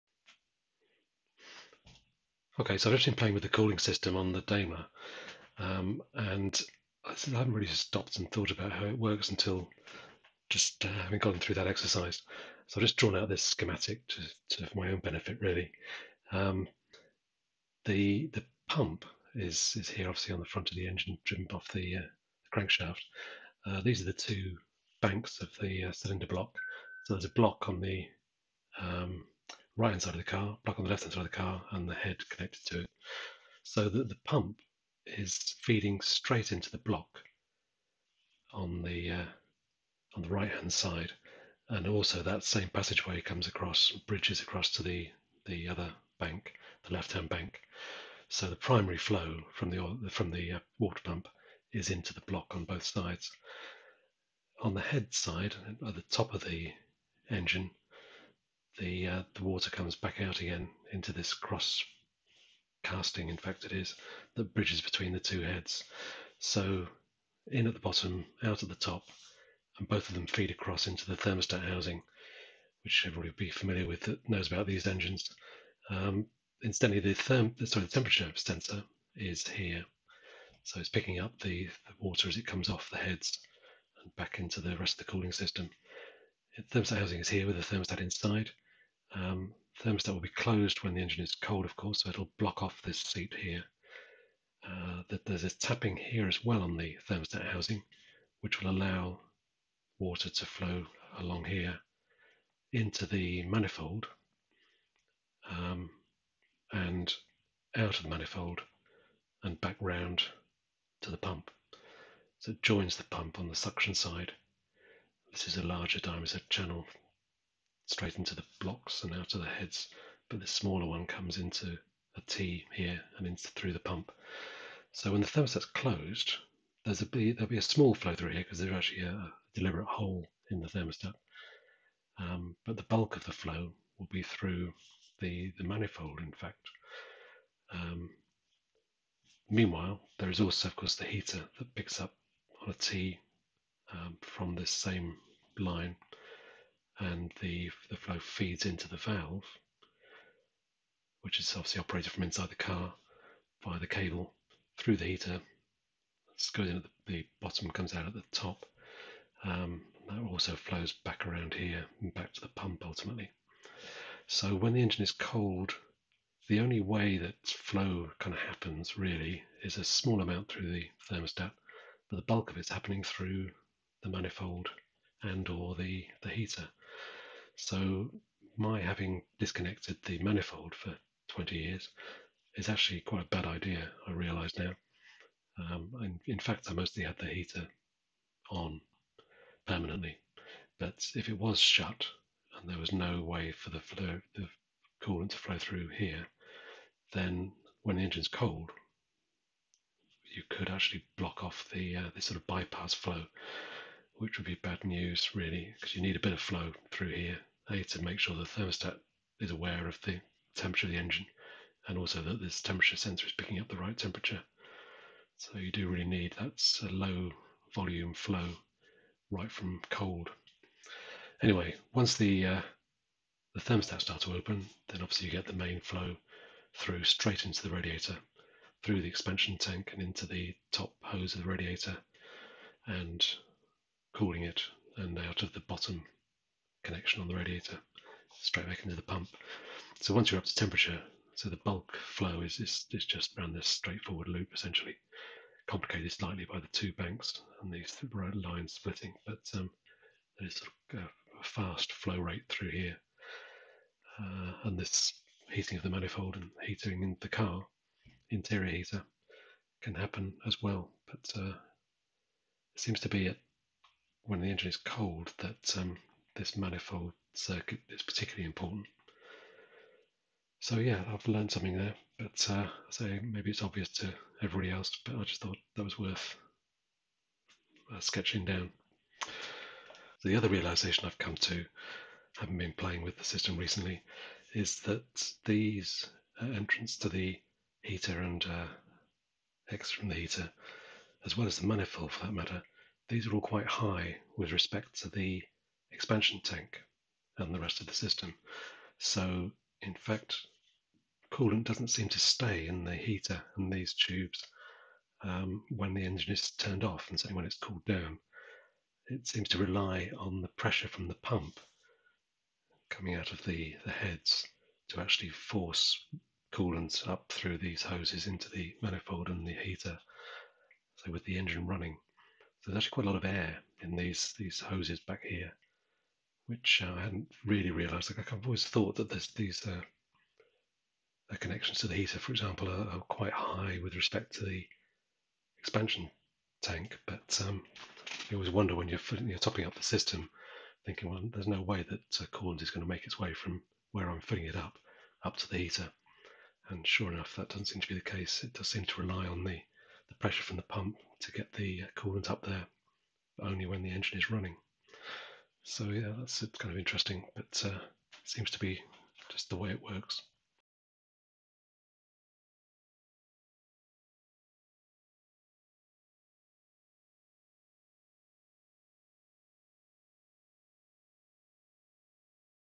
Daimler V8 Cooling Schematic
Having recently played with the cooling system on my SP250, here's an overview of the system layout